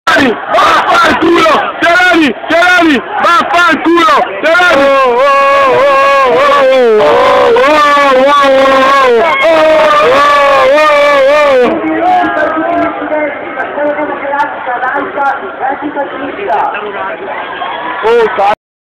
manifestazione contro fioroni a bergamo
migliaia di studenti di bergamo e della val seriana scioperano contro fioroni e contro il dm 80